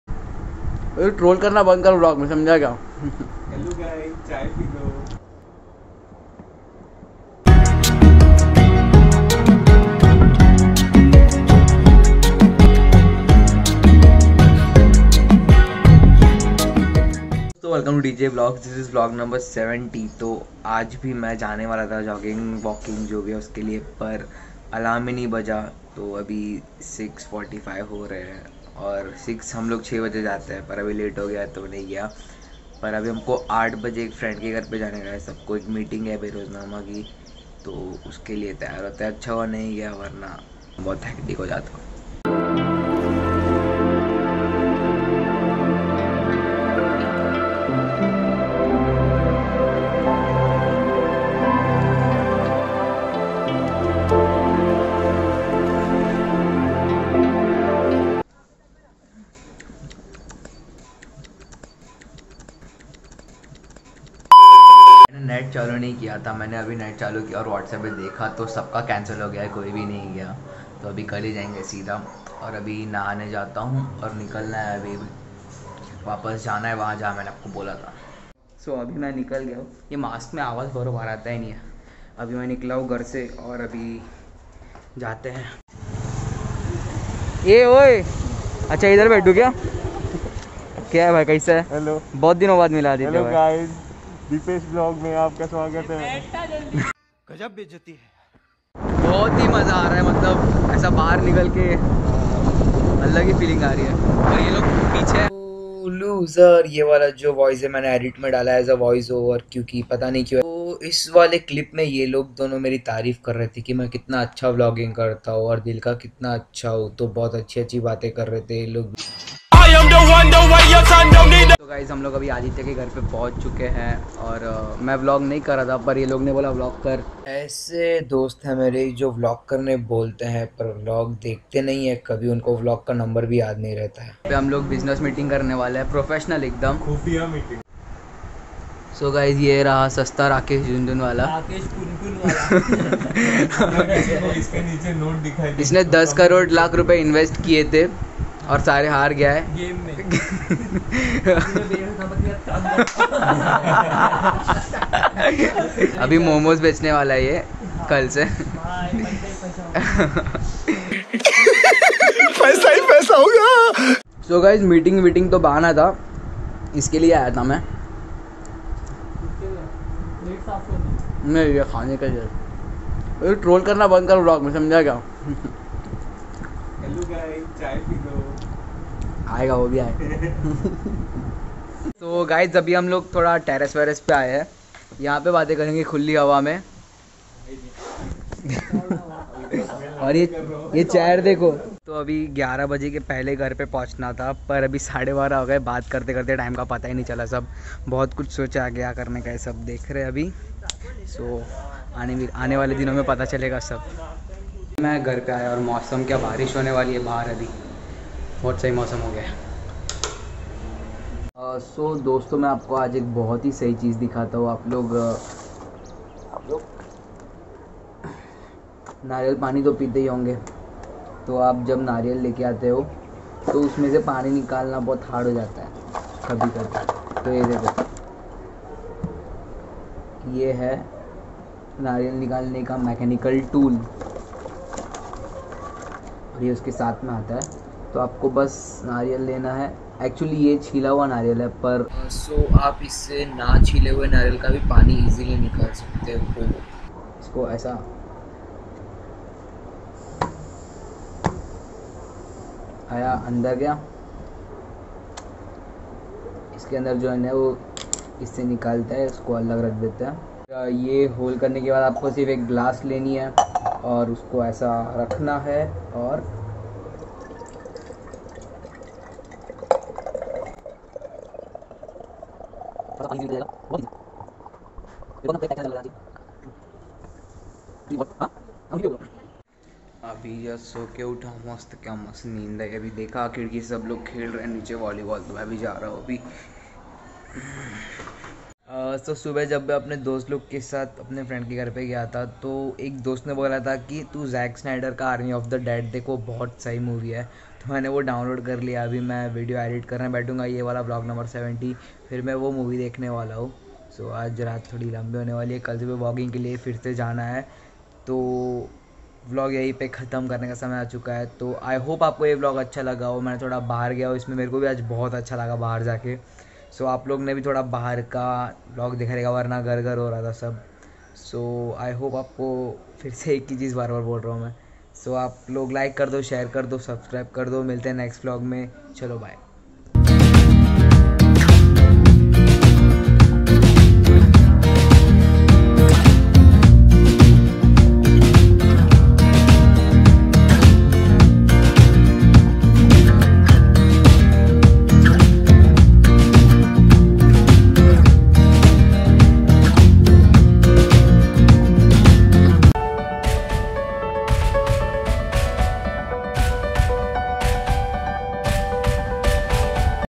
They Are making Fun of Me In My Own Vlog || Vlog 70 || DJ Vlogs
we all met after a very long time (Professionally). So couldn't leave this chance and so I vlogged. Also, in this vlog i have shown a tool that can help you remove coconut water from coconut very easily.